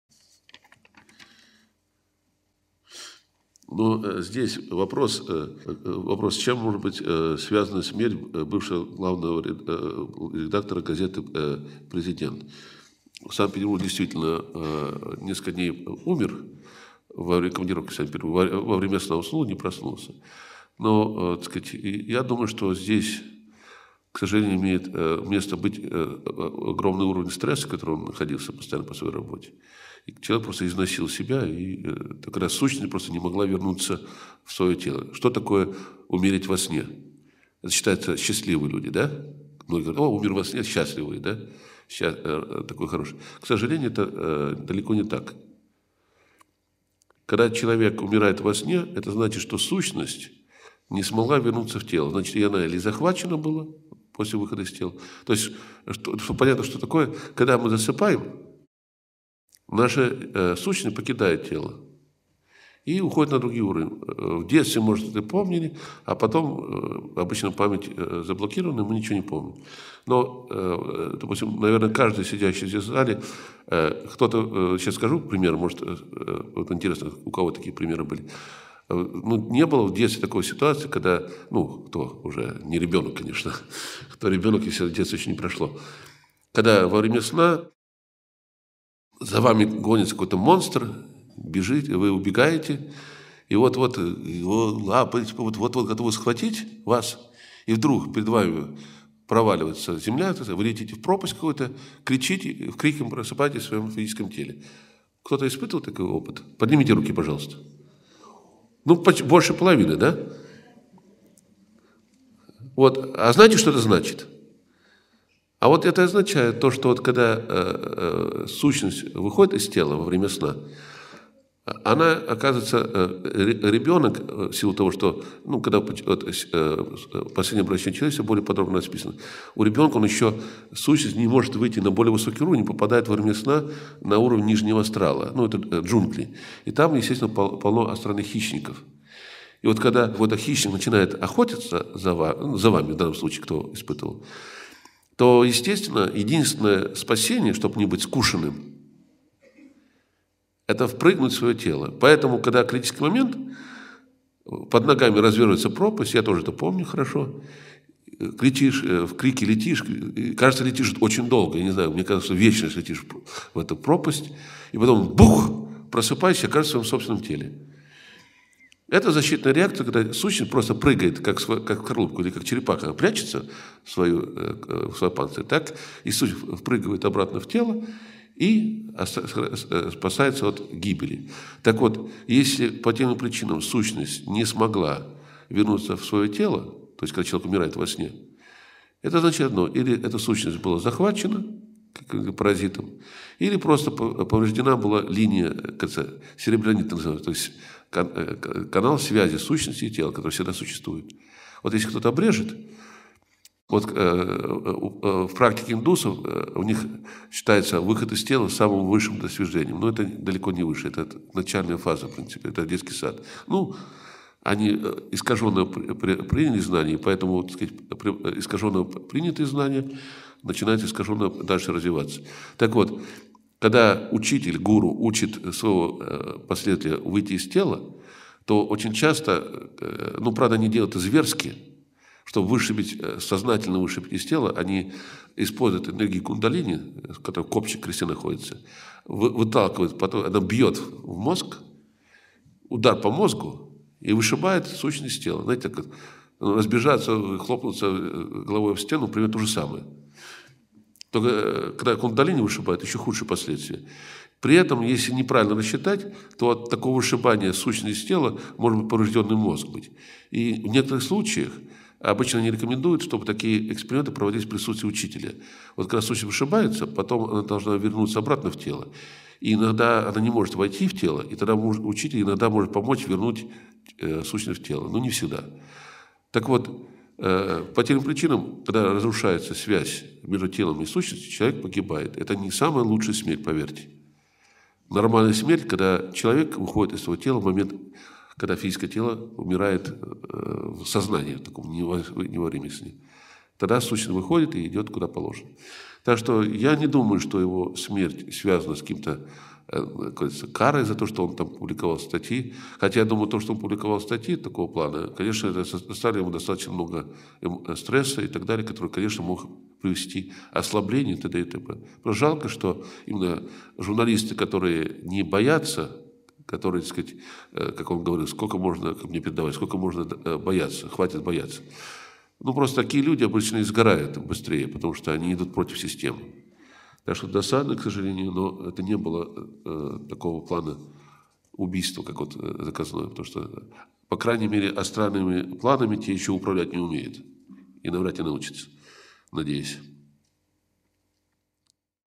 Kas vyksta su siela miegant? Mirtis miegant. Kundalini [NVL_LT]
Laikraščio „Prezident" buvusio redaktoriaus mirtis;
Kas vyksta su siela miegant. Mirtis miegant;
Meditacija pasitelkus Kundalini ir sielos "išmetimas" iš kūno.

daugiau informacijos filme "Siela gyvenimo ir mirties rate" [LT subtitrai]